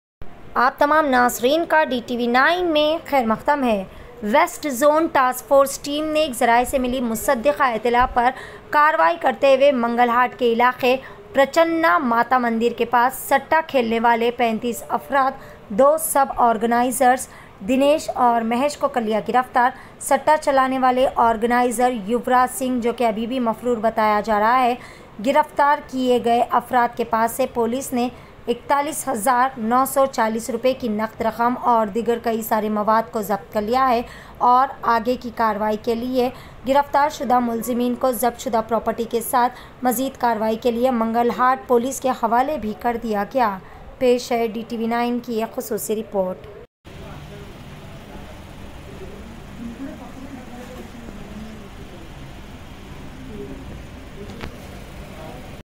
WestZone Taskforce TEAM' ne Mandir Ke Pas Satta Khelne Wale 35 Afraad Ko Kiya Giraftaar..
WestZone Taskforce Ne Ek Ettella Per Mangalhat Ke Elaqe Prachanda Mata Mandir Ke Pas Satta Khelne Wale 35 Afraad 2 Sub-Organisers Dinesh Aur Mahesh Ko Kiya Giraftaar..

Satta Chalane Wale Organiser Yuvraj Singh Farar Bataya Gaya Hai..

Giraftaar Kiye Gaye Afraad Ke Pas Se Police Ne  Rs. 41940/- Cash Aur Doosre Saman Kiya Zabt...

Aur Agay Ki Karwai Ke Liye Mangalhat Police Ke Hawale Kardiya...